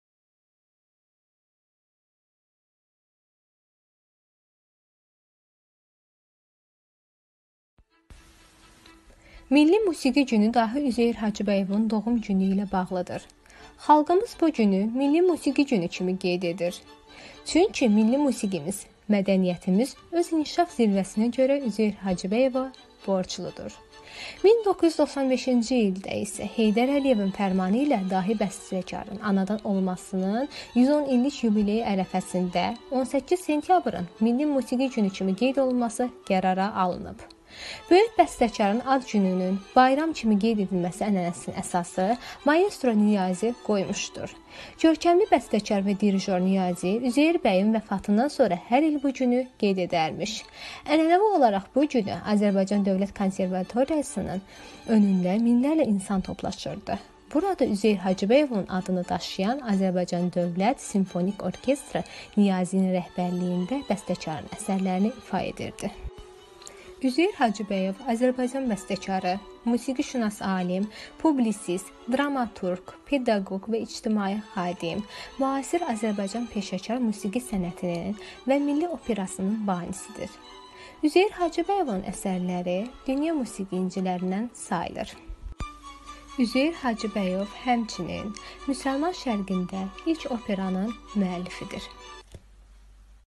Tərtər rayon MKS - nin Qarağacı kənd kitabxana filialı
Bərdə Regional Mədəniyyət İdarəsi Tərtər rayon MKS - nin Qarağacı kənd kitabxana filialı Milli Musiqi Günü ilə   əlaqədar videoçarx hazırlayıb.